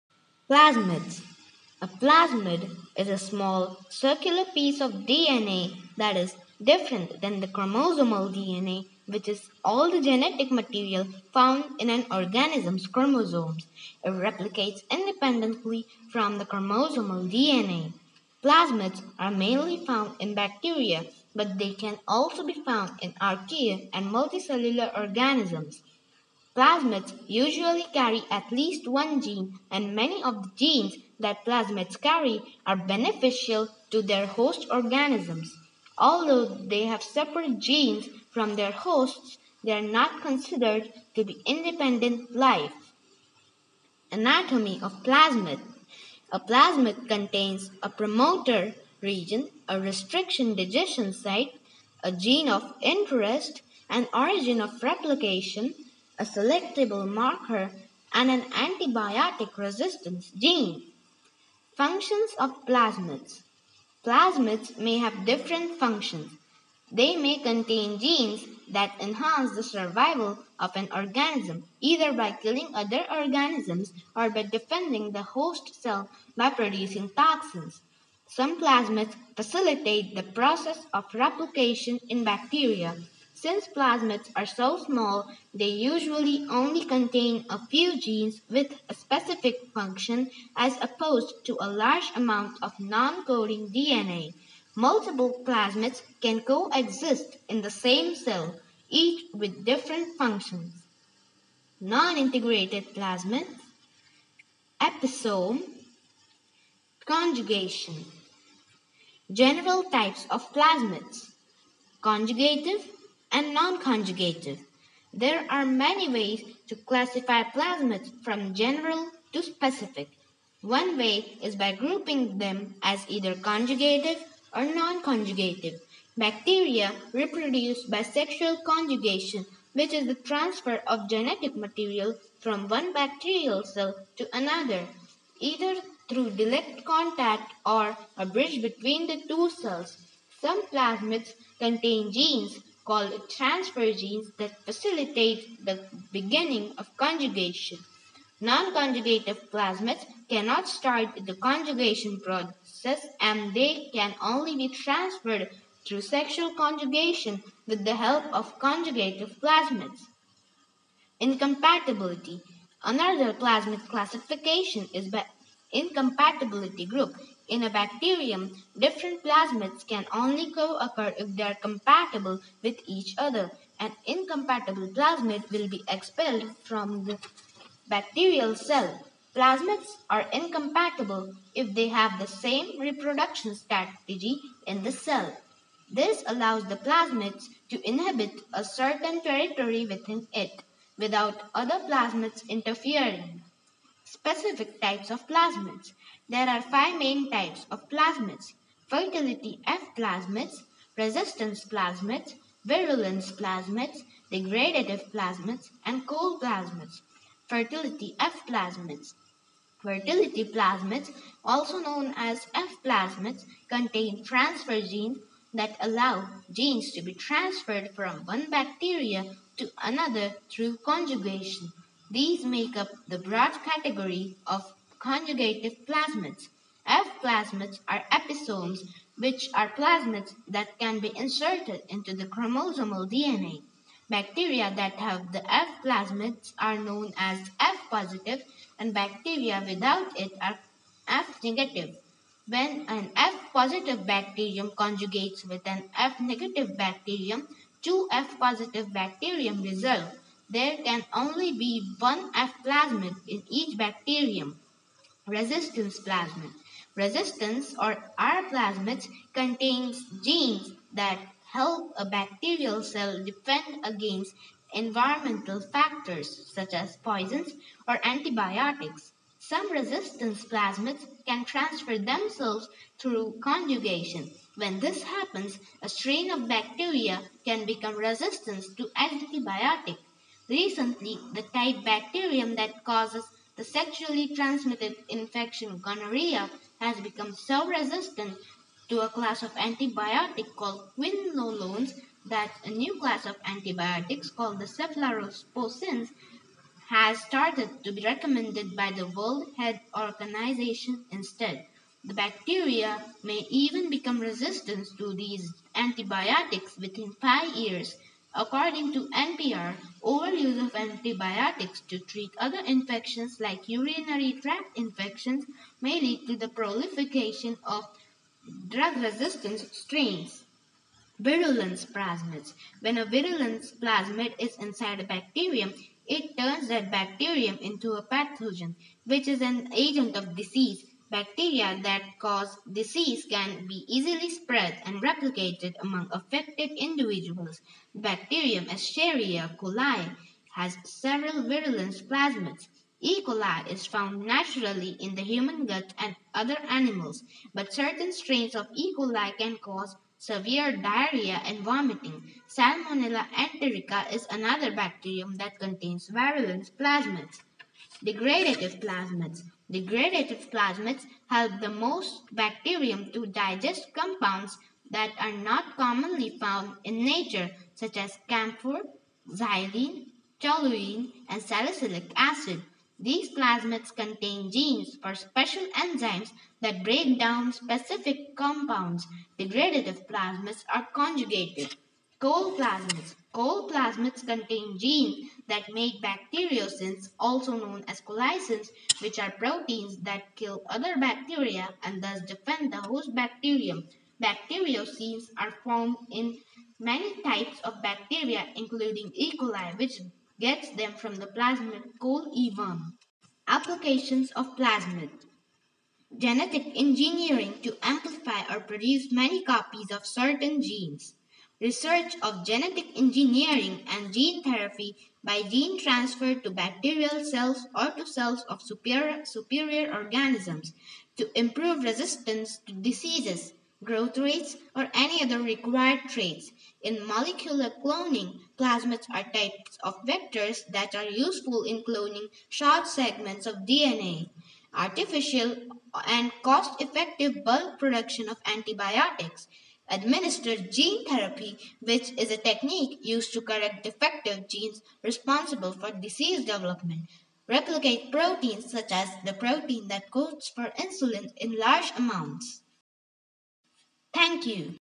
Lecture 3: Plasmids
Lecture 3
Plasmids

What are Plasmids?
Anatomy of Plasmids
Functions of Plasmids
Non-Integrated Plasmid
Episome
Conjugation
General Types of Plasmids
    ◦ Conjugative and Non-Conjugative
    ◦ Incompatibility
Specific Types of Plasmids
    ◦ Fertility F-Plasmids
    ◦ Resistance Plasmids
    ◦ Virulence (Ti) Plasmids
    ◦ Degradative Plasmids
    ◦ Col Plasmids
Applications of Plasmids

Dhruva's Web